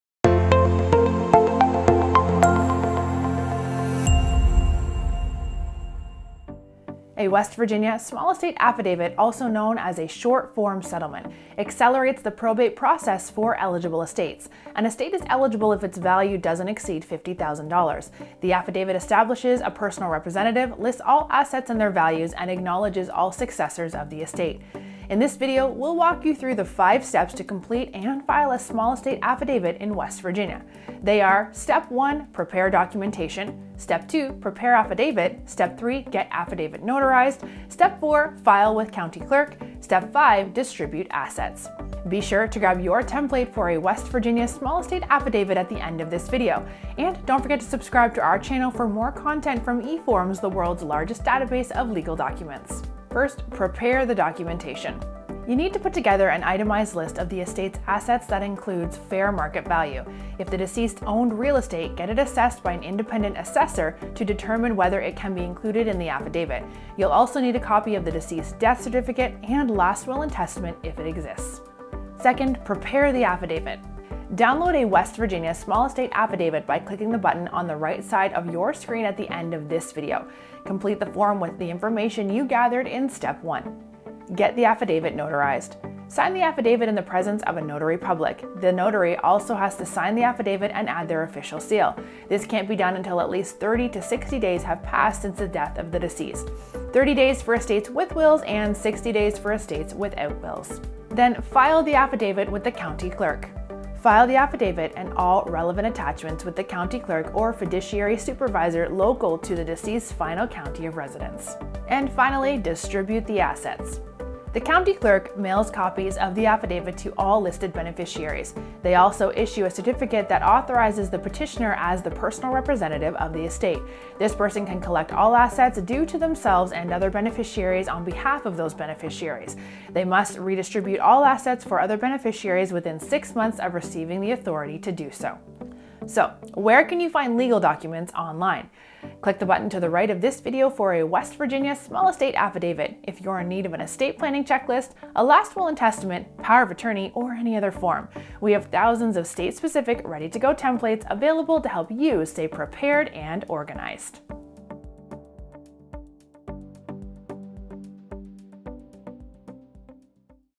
West Virginia Small Estate Affidavit - EXPLAINED
A West Virginia small estate affidavit is designed for residents of Wisconsin to speed up or skip probate if they are the beneficiary of a small estate. This form allows the beneficiary to collect the deceased’s assets directly from the parties that control them.

Our video explains the process of completing and filing this small estate affidavit in West Virginia.

See the West Virginia Small Estates Act for the applicable statutes.

0:00 Intro:
0:31 We'll Discuss:
0:54 Prepare The Documentation:
1:12 Prepare The Affidavit:
1:24 Get Affidavit Notarized:
1:42 File With County Clerk:
1:51 Distribute Assets:
2:16 Where Can You Find Legal Documents Online: